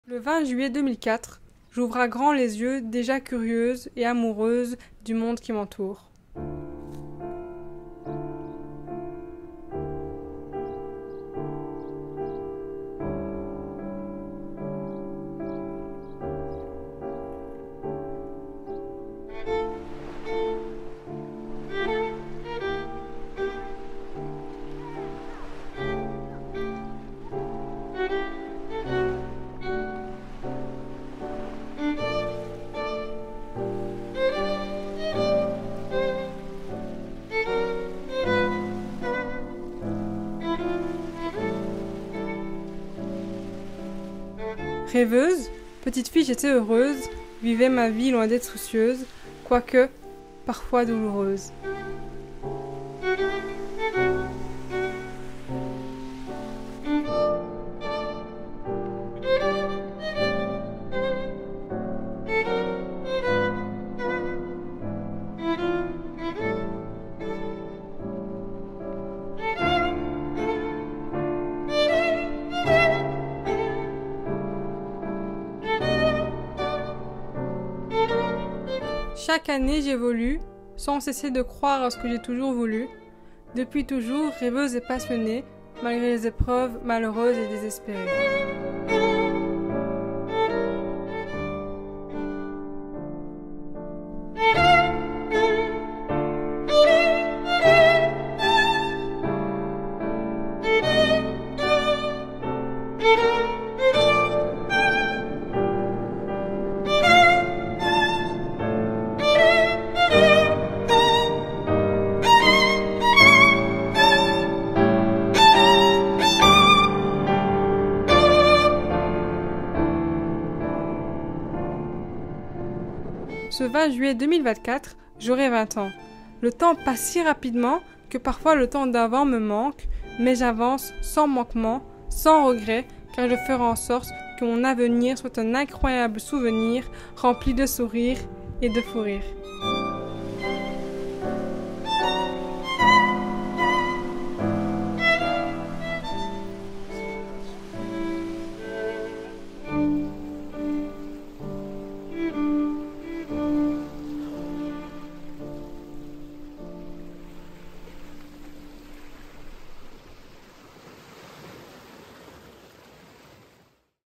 MINI FILM POUR MES 20 ANS
20 ans, c'est quelque chose... C'est on va dire un ARC qui s'ouvre et un autre qui se referme donc c'est à la fois flippant et excitant! Pour marquer ce moment, faire une vidéo spéciale me tenait à coeur donc voilà un mini film un peu poétique sur mes 20 ans :)

.filmé par moi et mon frérot (merci à lui)
.filmé avec Sony ZV-1 et iPhone 13 Pro (de mon frérot)
.filmé pendant la 1ère semaine de juillet